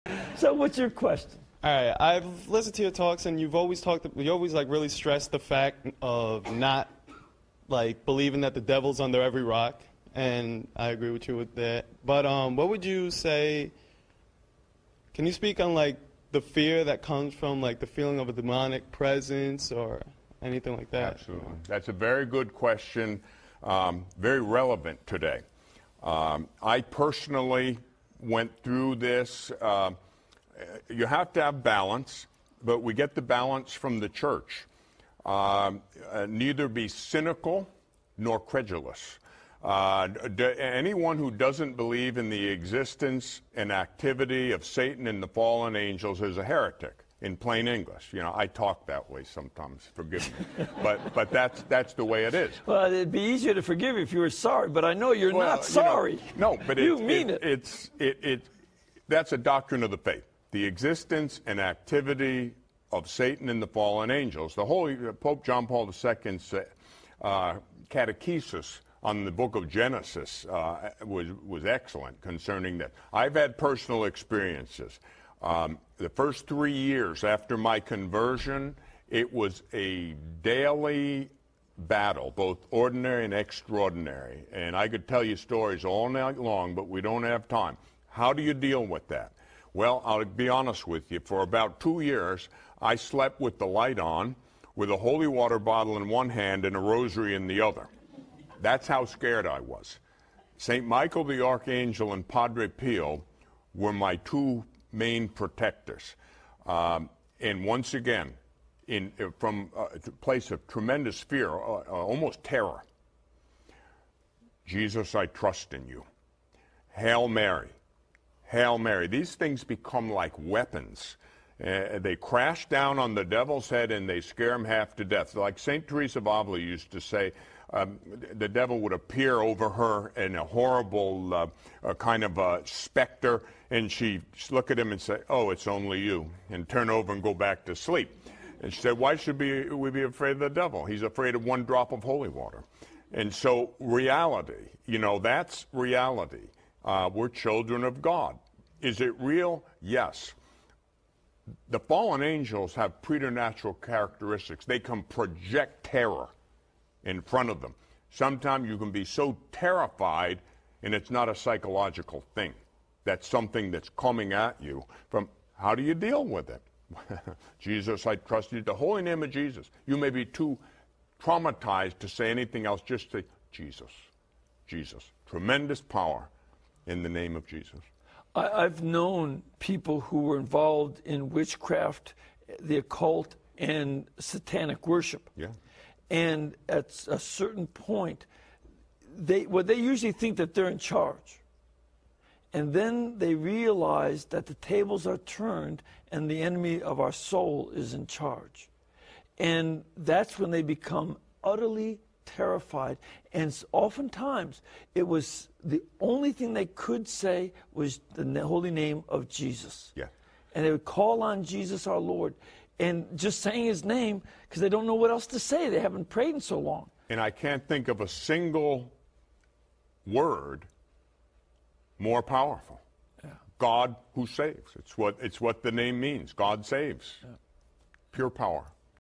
St. Michael the Archangel and St Padre Pio were my two main protectors...
Anyone who doesn't believe in the existence and activity of Satan and the fallen angels is a heretic, in plain English...
That's a doctrine of the faith. The Holy Father, Pope John Paul 11 Catechesis on the Book of Genesis was excellent concerning this. I've had personal experiences, the first three years after my conversation it was a daily battle, both ordinary and extraordinary, I could tell you stories all night long but we don't have time. How do you deal with it...Well I'll be honest with you, for two years I slept with the light on, with holy water in one hand and the rosary in the other, that's how scared I was. St. Michael the Archangel and St Padre Pio were my two main protectors...
And once again a place of tremendous fear, almost terror, Jesus I trust in you, Hail Mary, Hail Mary. These things become like weapons, they crash down on the devils head, and they scare him half to death...